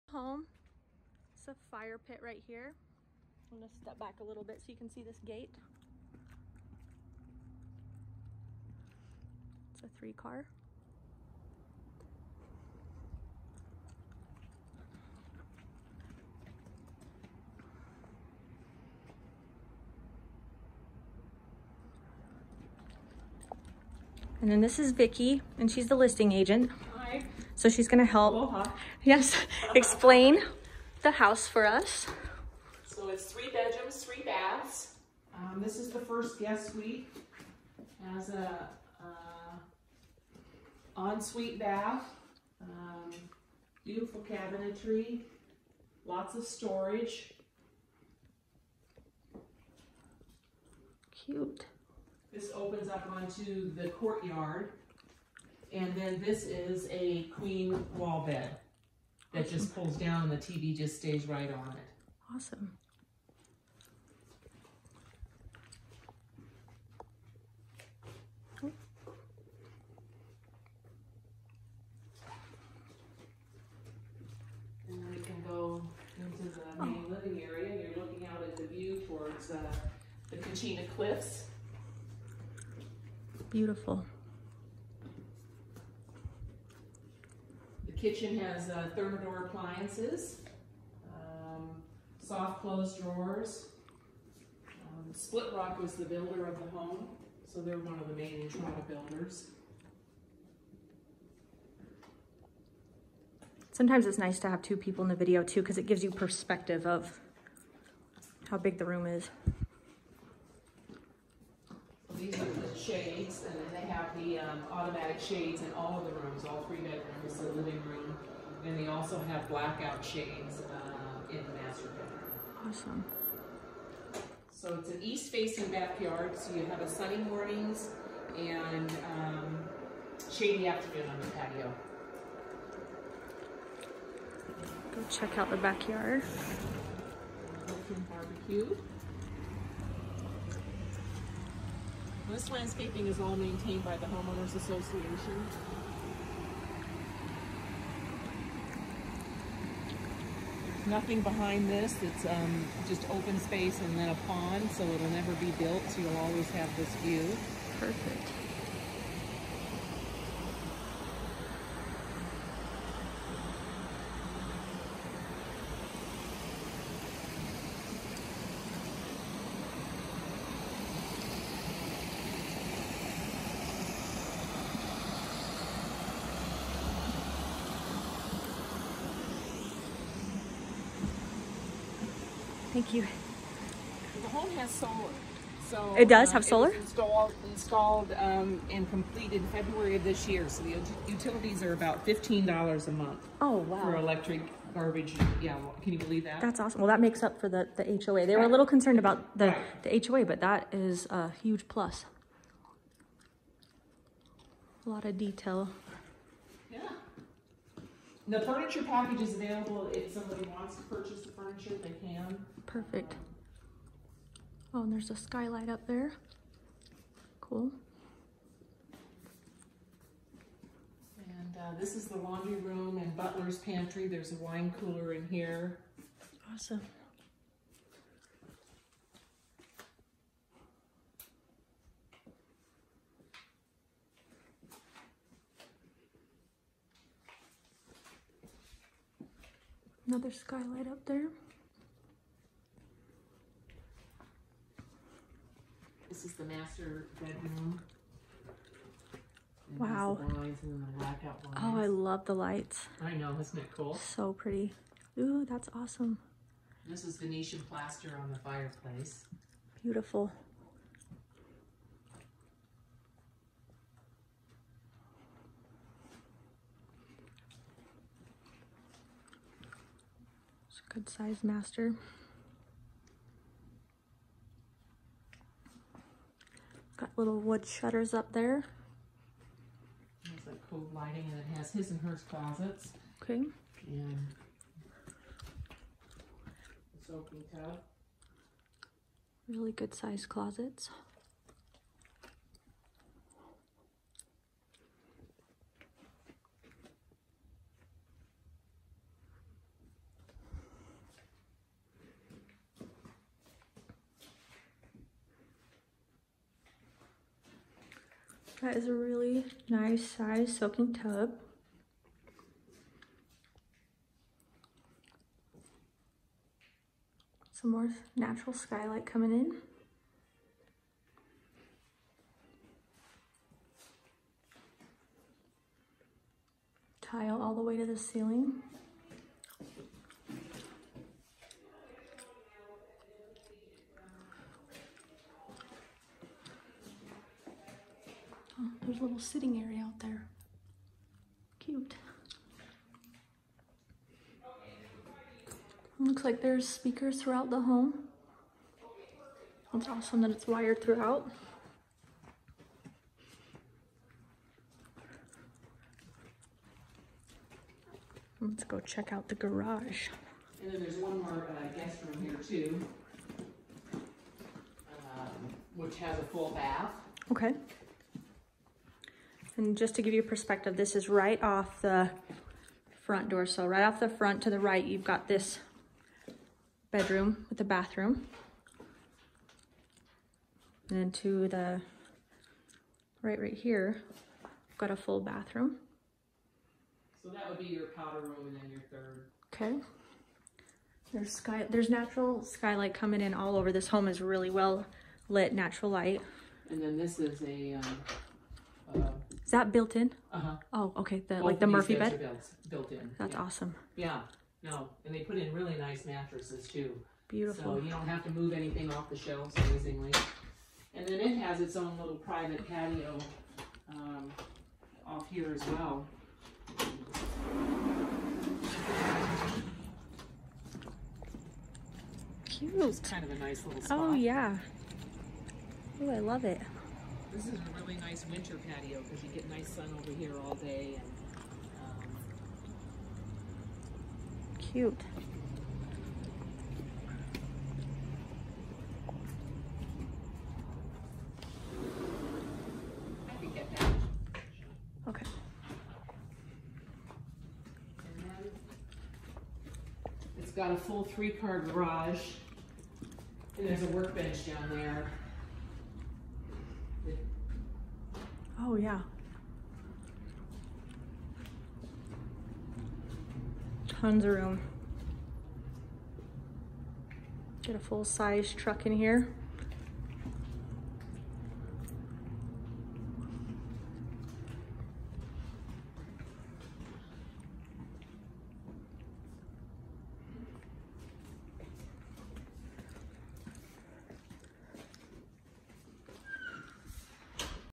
Ivins, Utah | Southern Utah Luxury Home Tour
Property tour of a beautiful luxury southern Utah home in a gated community - Entrada.

🎦Watch videos about best places to live in Southern Utah

🎦Watch videos about the custom Parade homes in St George